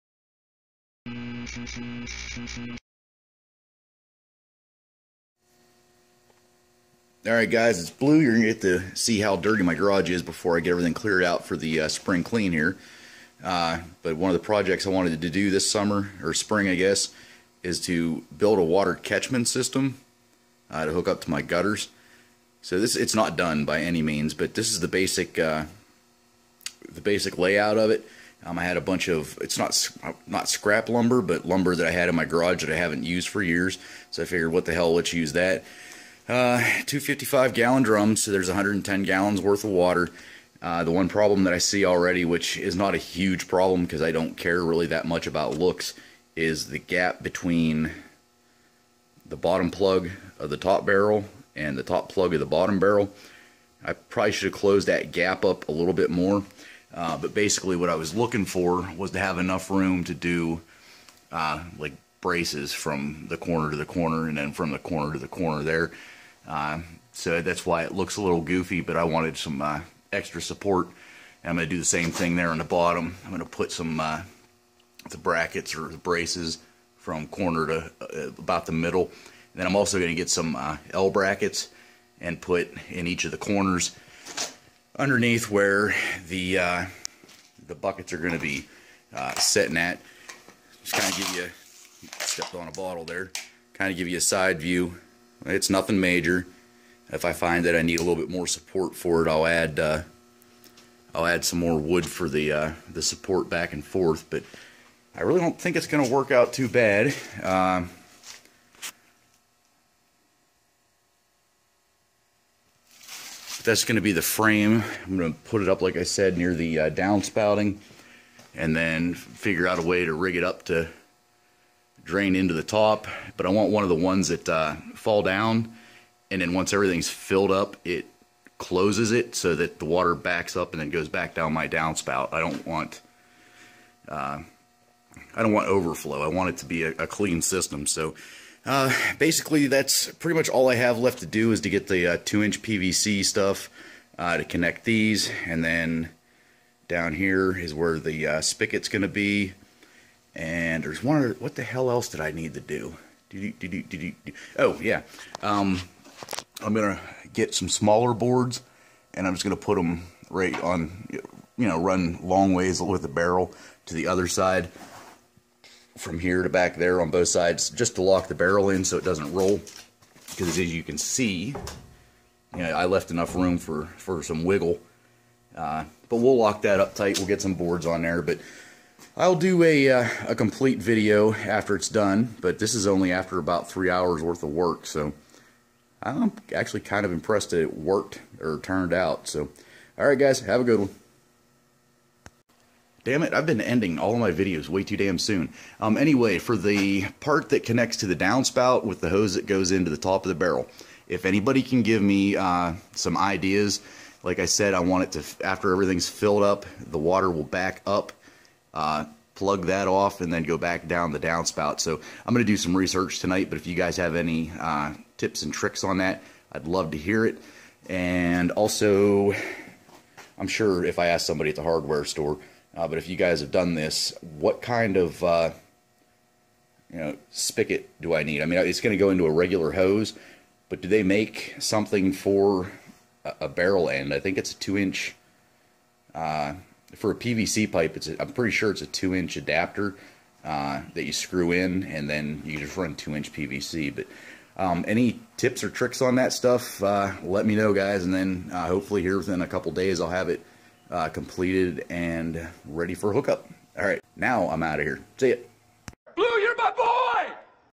Water Catchment System, Part 1. Building The Blue Barrel Frame
My first attempt at a water catchment system. This is not a how to, more of a this is what I am doing... will do a few updates along the way. Any ideas are appreciated.